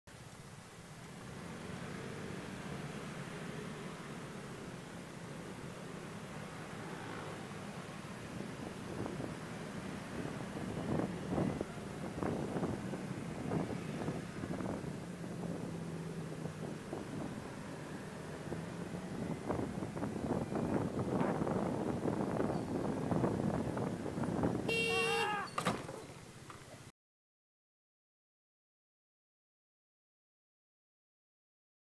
原潑這吶喊聲挺抓馬的（開聲音）
原標題：前方突然煞車，我按煞車來不及撞到